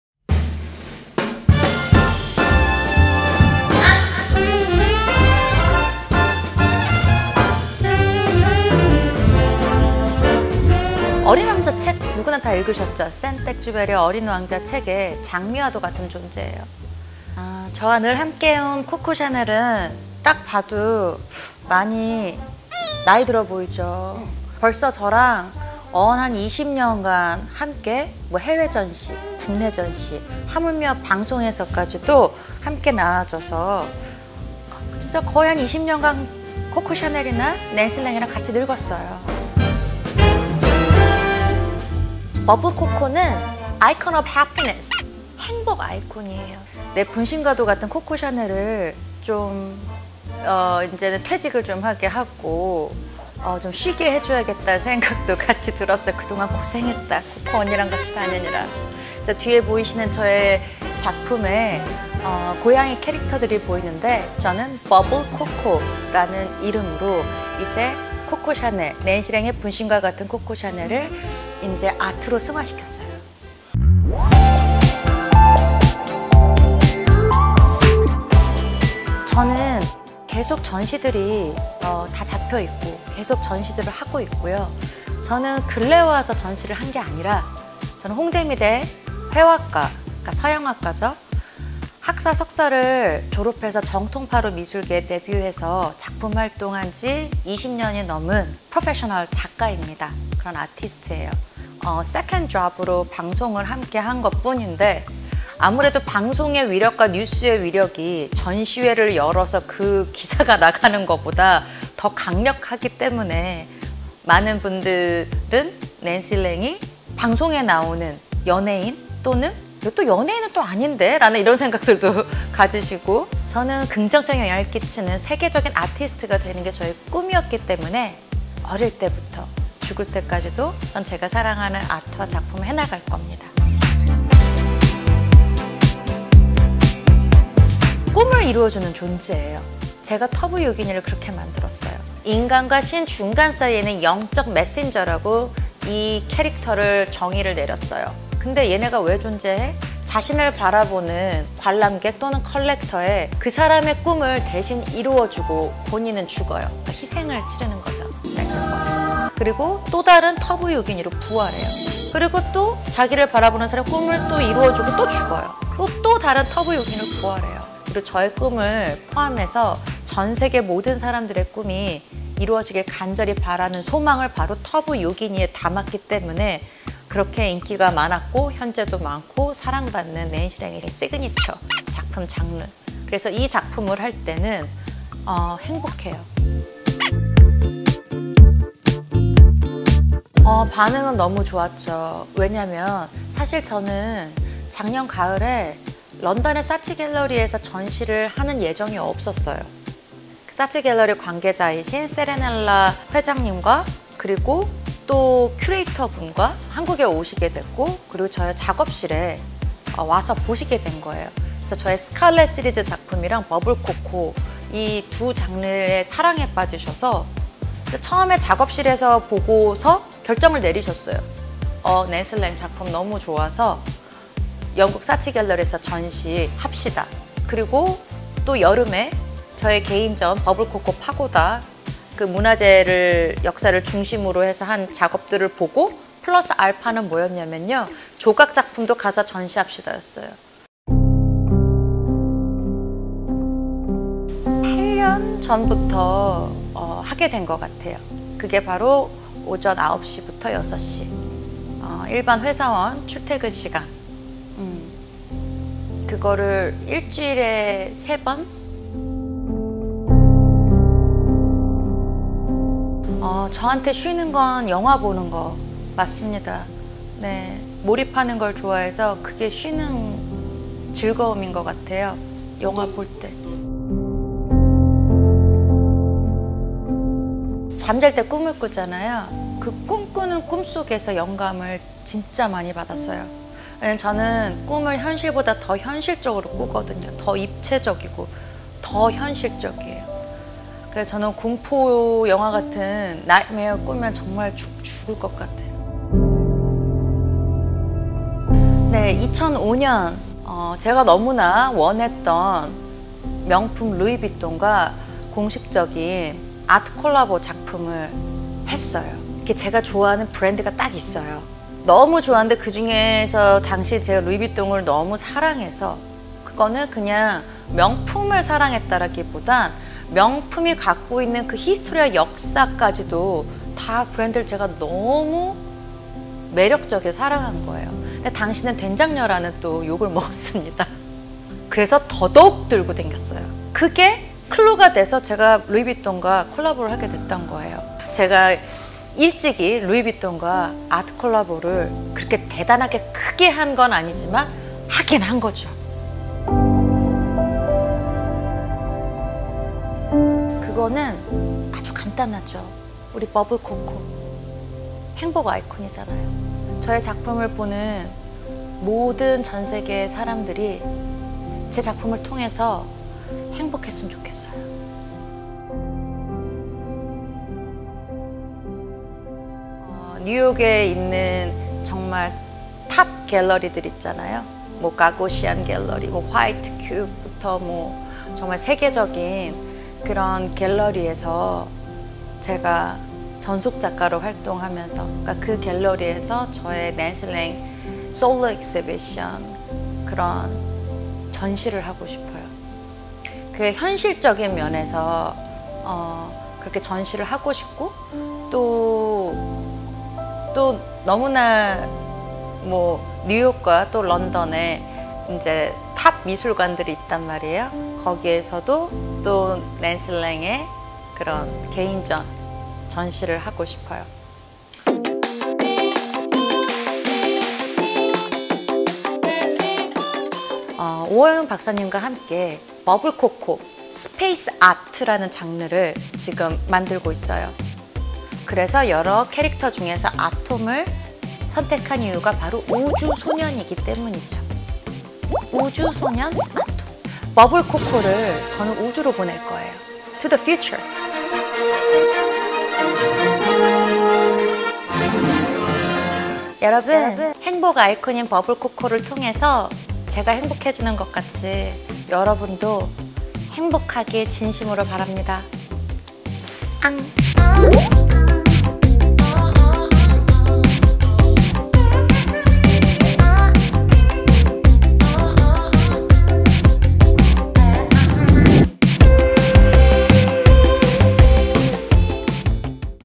'SPACE ART' 팝아티스트 낸시랭 작가 인터뷰 / 'SPACE ART' Interview with Pop Artist Nancy Lang
"'버블코코'를 우주로 보내는 목표를 이루기 위해 작년부터 누리호 큐브 위성 연구팀과 작업을 진행 중이다"